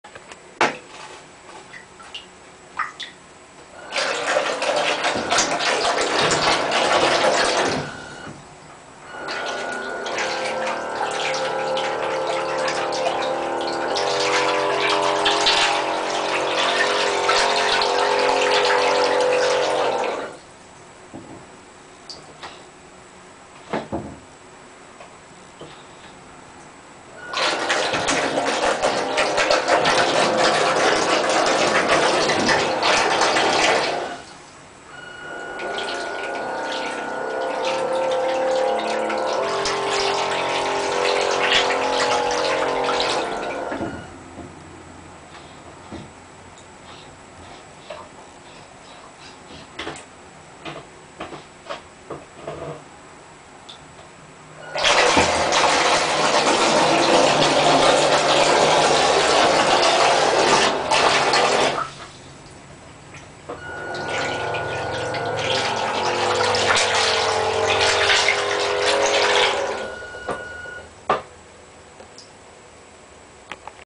Homemade RC Submarine
Sub made from basic materials, pvc pipe, cup with silicon seal from a dollar store, knex, legos, a bilge pump and lots of goop glue